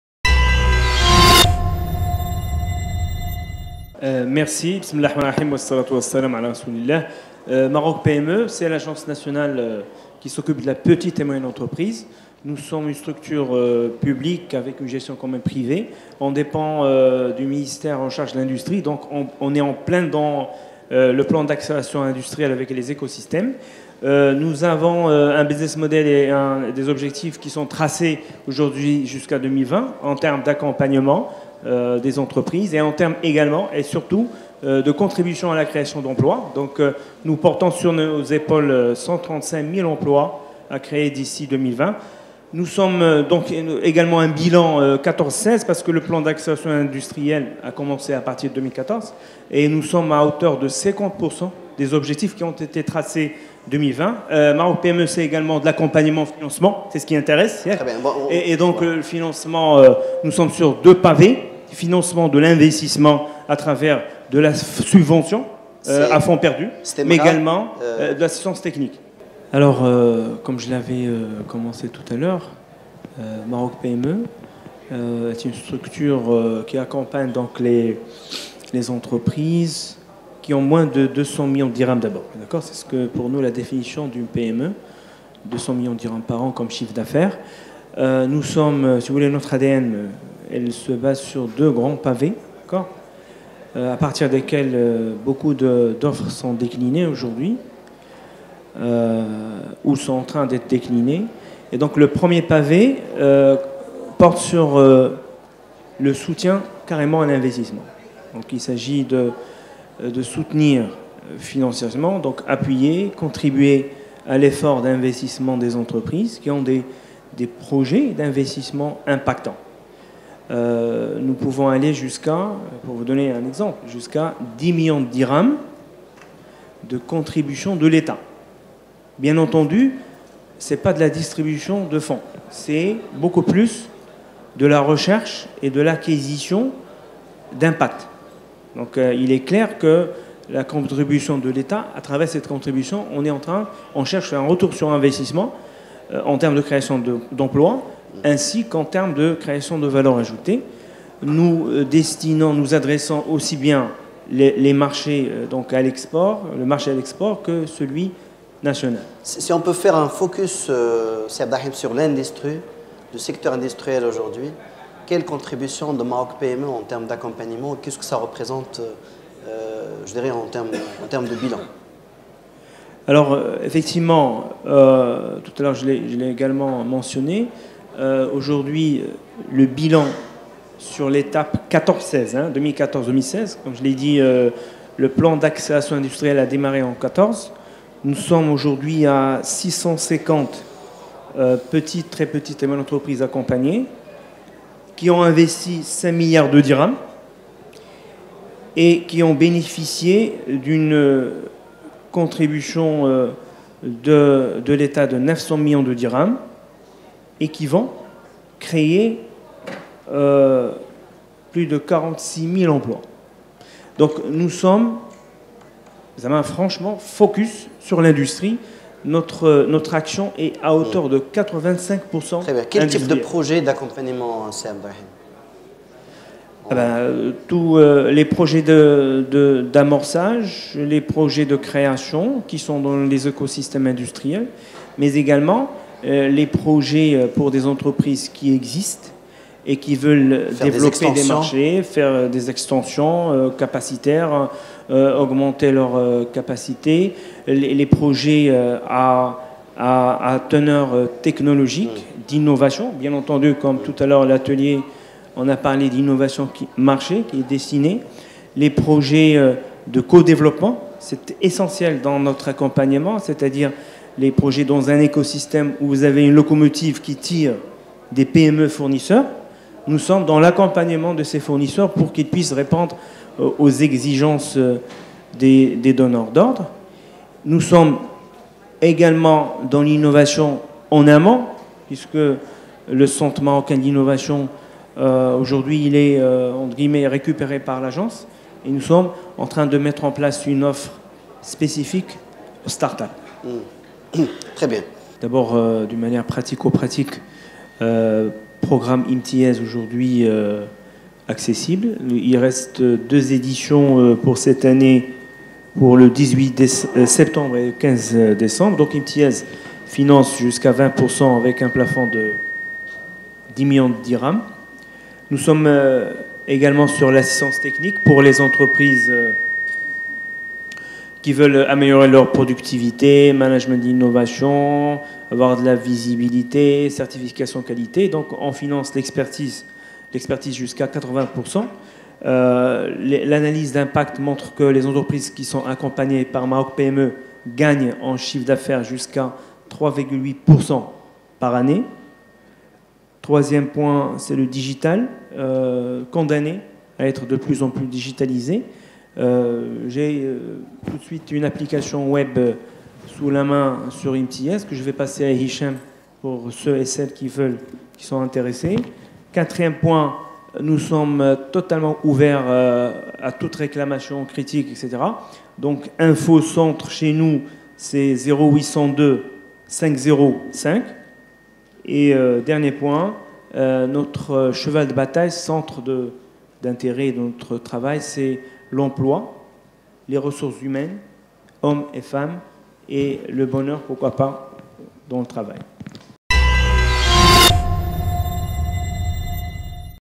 Abderrahim Aquesbi, L’accompagnement des industriels, quelle offre ?
Abderrahim Aquesbi Directeur Général Adjoint de  l’Agence Nationale pour la Promotion des Petites et Moyennes Entreprises (ANPME)

INDUSTRIE DU MAROC MAGAZINE organise une fois par an un événement intitulé «INDUSTRY MEETING DAY», visant à débattre des thématiques spécifiques à l’industrie porteuse de développement et qui seront animées par des experts du domaine.
«INDUSTRY MEETING DAY» est ouverte à tous, chefs d’entreprises, experts, porteurs de projets…

 IDM BUSINESS TV